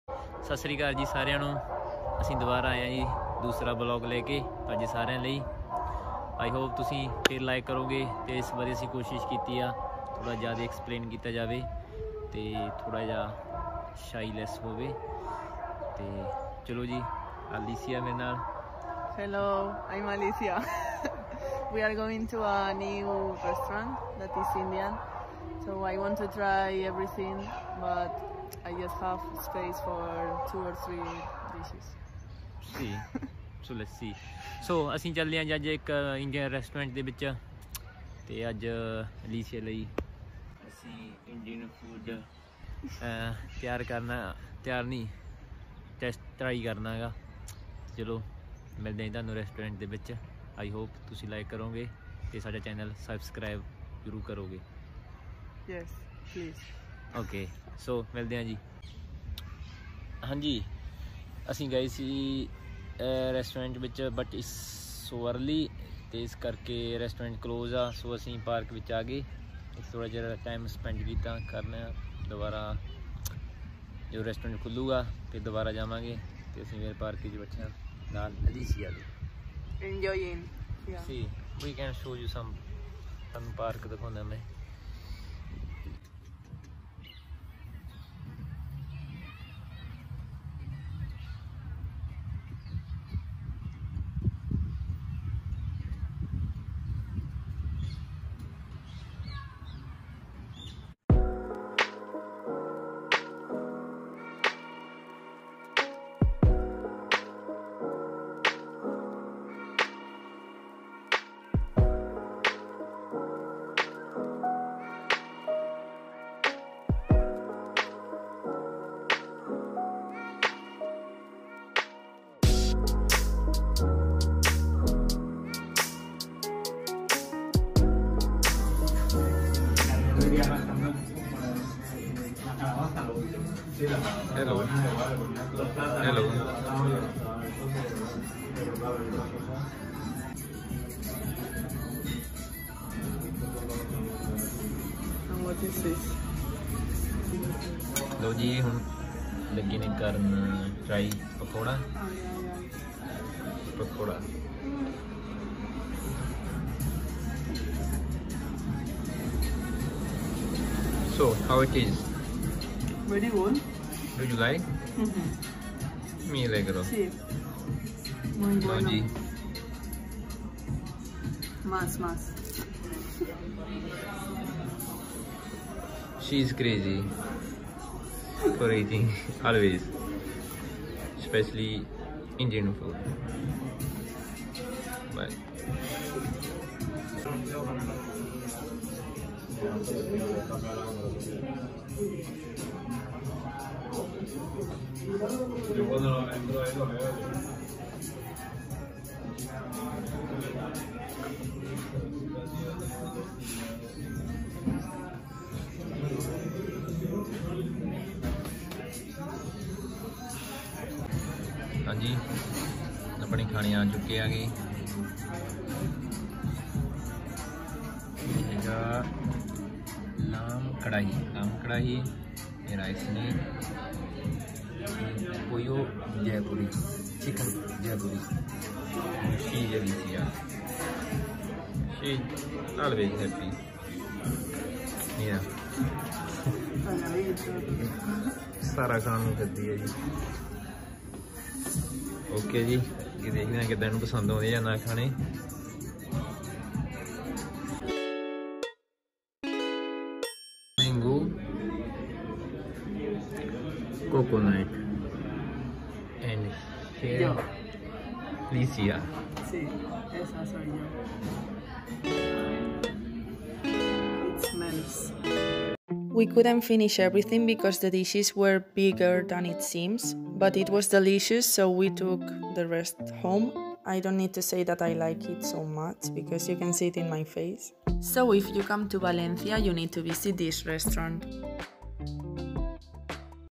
Third vlog || Trying Indian food 😋
In this video we decide to eat in an Indian restaurant after taking some rest in a park of Valencia. They gave so much food and it was super, you need to try it! 🤭